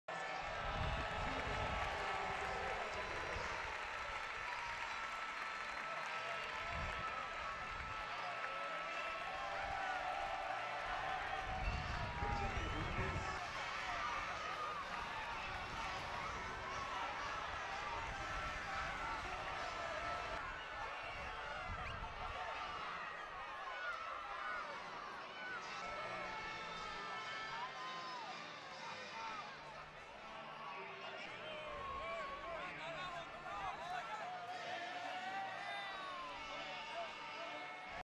UD Las Palmas campeón Trofeo Carranza 2017
El equipo canario ha logrado el histórico trofeo tras superar al Málaga por 2 a 0 que no pudo reeditar el título obtenido el año anterior